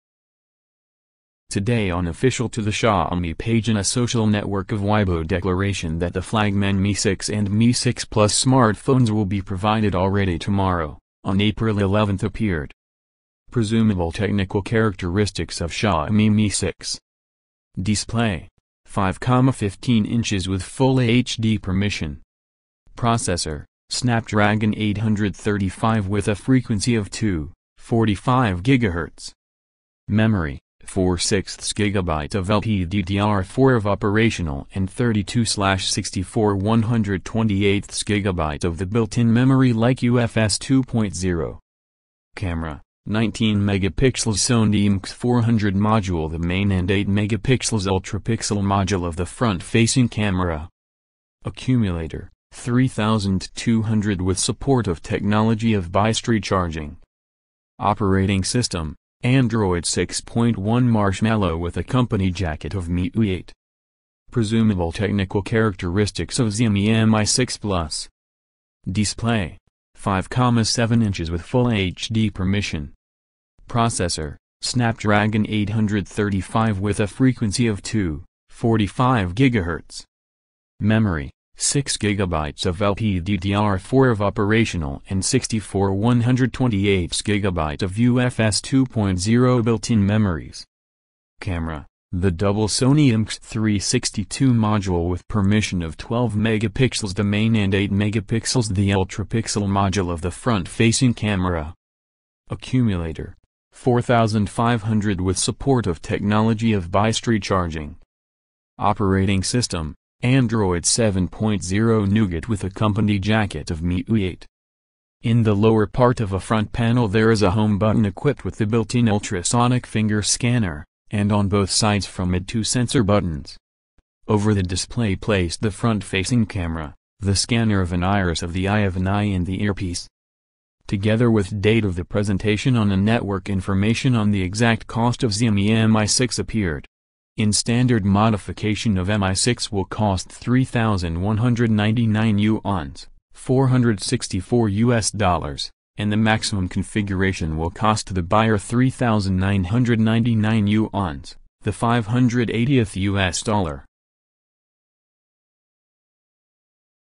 Xiaomi Mi6 & Mi6 Plus Specifications
Today on official to the Xiaomi page in a social network of Weibo declaration that the flagman Mi6 and Mi6 Plus smartphones will be provided already tomorrow, on April 11 appeared.

Presumable technical characteristics of Xiaomi Mi6:

Display: 5,15 inches with Full HD permission
Processor: Snapdragon 835 with a frequency of 2,45 GHz
Memory: 4/6 GB of LPDDR4 of operational and 32/64/128 GB of the built-in memory like UFS 2.0
Camera: 19 Megapixels Sony IMX400 module the main and 8 Megapixels Ultrapixel module of the front-facing camera
Accumulator: 3200 мАч with support of technology of bystry charging
Operating system: Android 6.1 Marshmallow with a company jacket of MIUI 8
Presumable technical characteristics of Xiaomi Mi6 Plus:
Display: 5,7 inches with Full HD permission
Memory: 6 GB of LPDDR4 of operational and 64/128 GB of UFS 2.0 built-in memories
Camera: the double Sony IMX362 module with permission of 12 Megapixels the main and 8 Megapixels the Ultrapixel module of the front-facing camera
Accumulator: 4500 мАч with support of technology of bystry charging
Operating system: Android 7.0 Nougat with a company jacket of MIUI 8
In the lower part of a front panel there is a Home button equipped with the built-in ultrasonic finger scanner, and on both sides from it two sensor buttons. Over the display placed the front-facing camera, the scanner of an iris of the eye of an eye and the earpiece.

Together with date of the presentation on a network information on the exact cost of Xiaomi Mi6 appeared. In standard modification of Mi6 will cost 3199 yuans (464 US dollars), and the maximum configuration will cost the buyer 3999 yuans (580th US dollar).